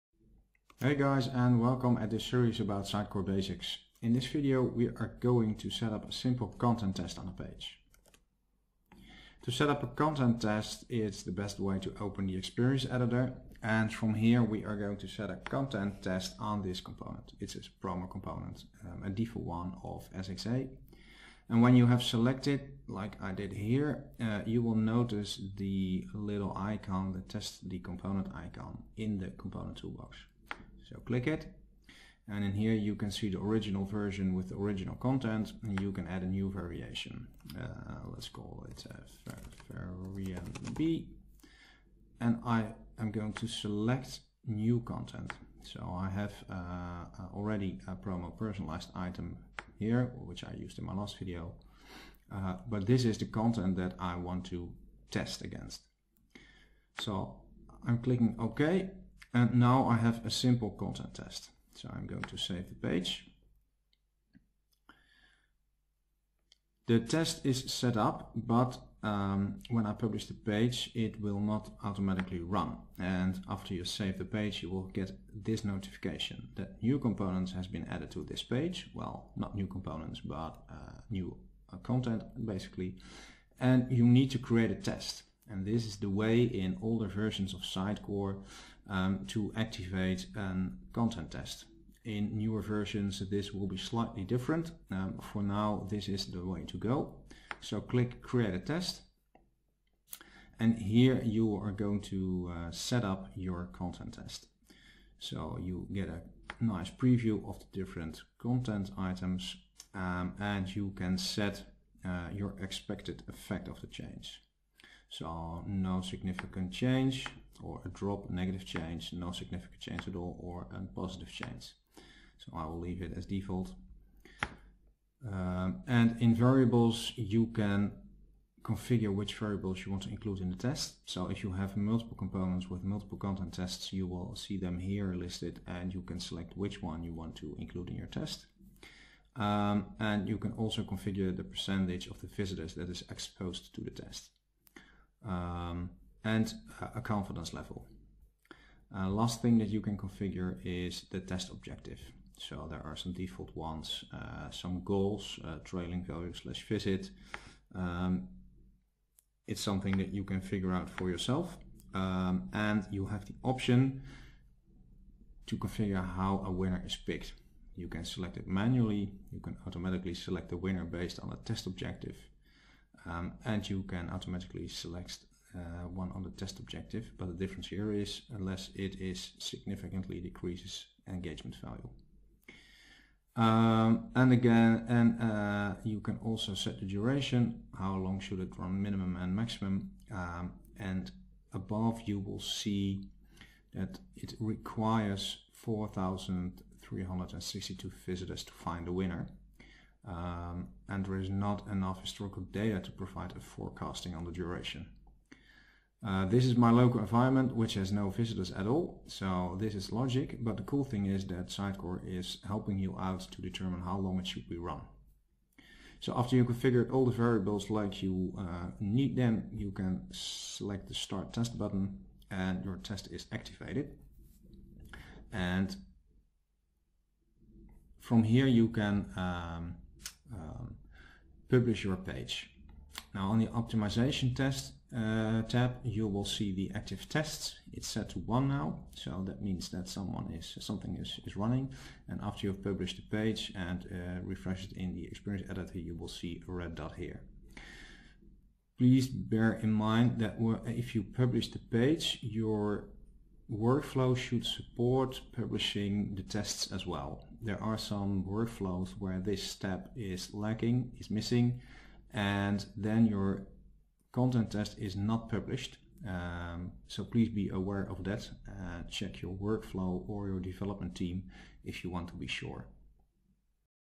27 - Sitecore Basics Series - Add a content test
Learn how to setup a simple content test for a componentn on a page.